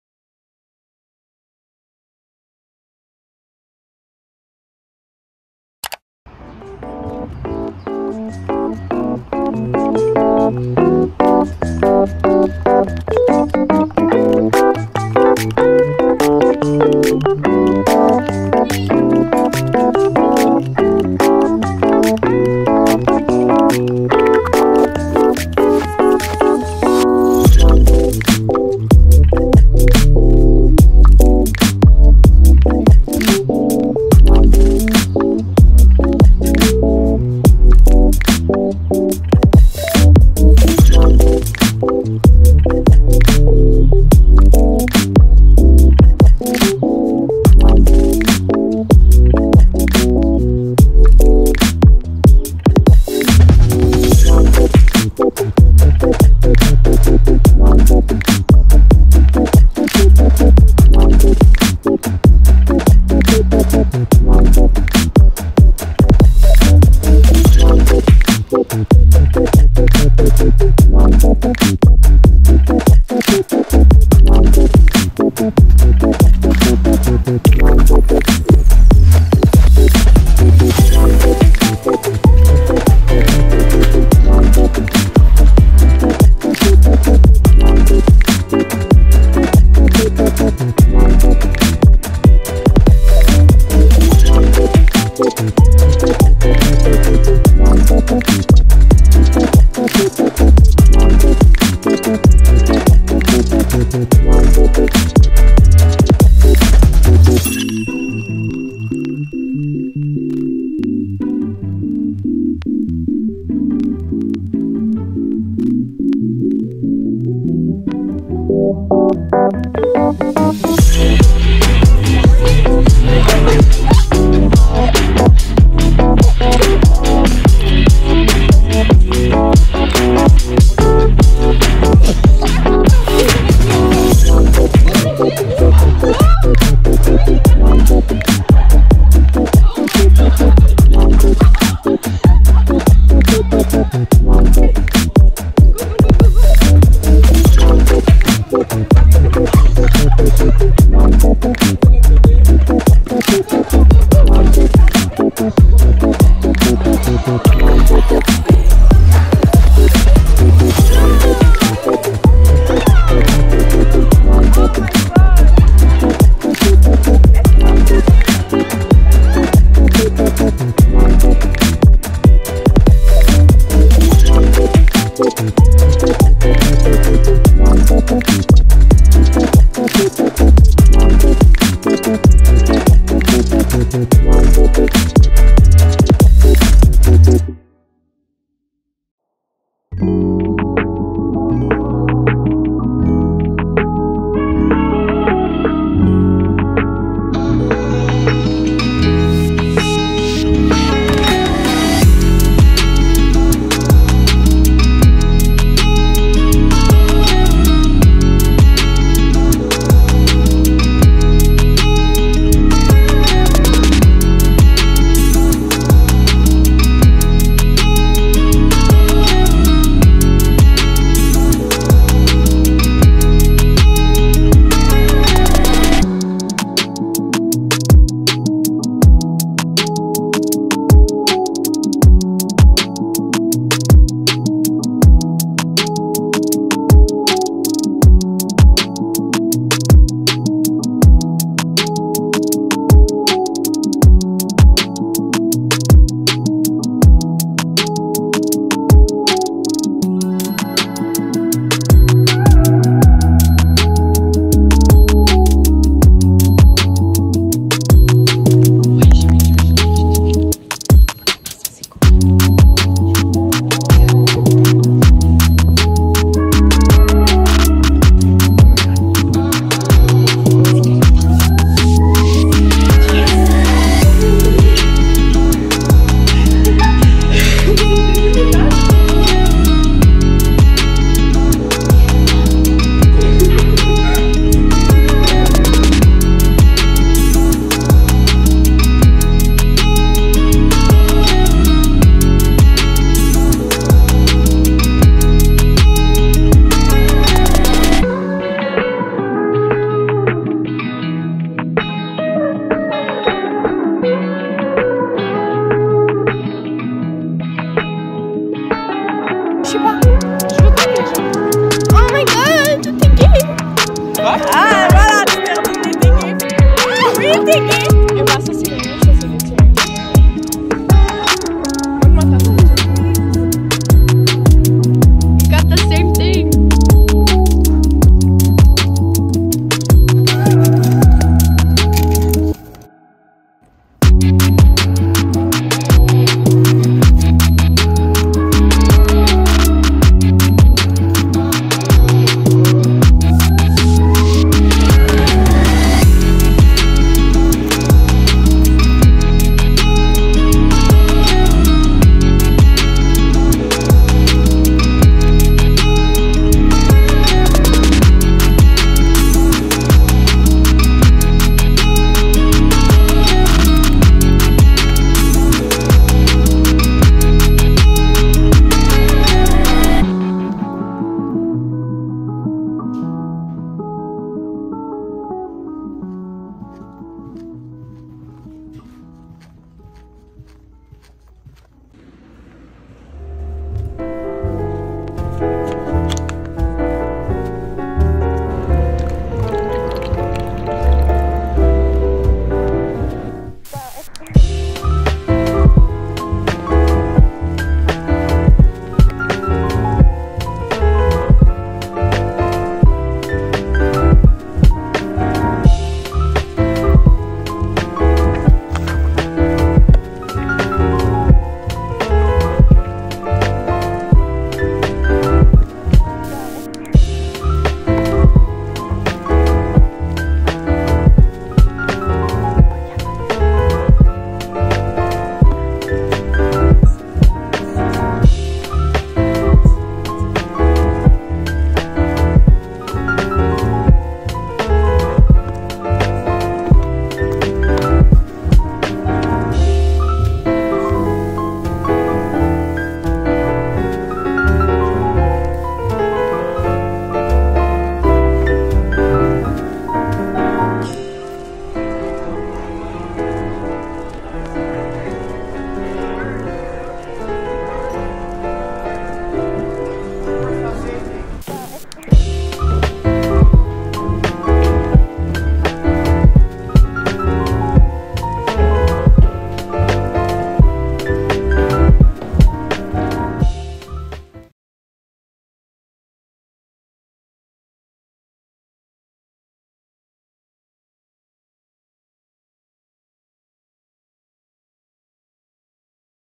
Daily vlog: going at the biggest library in Canada ft. My friends | StrawberryBoba
❤️❤️❤️❤️❤️❤️❤️❤️❤️❤️❤️❤️❤️❤️Hey friends!

Today I went to the Grande Bibliothèque de Montréal aka the biggest library in Canada!

 It was so exhausting but we had a lot of fun 😂

If you live in Canada, you should definitely check it out or you haven’t already ❤️

Editing: VLLO
Camera: iPhone 13 Pro
Age: 14
Country: Canada
My nationality: 🇲🇩🇨🇦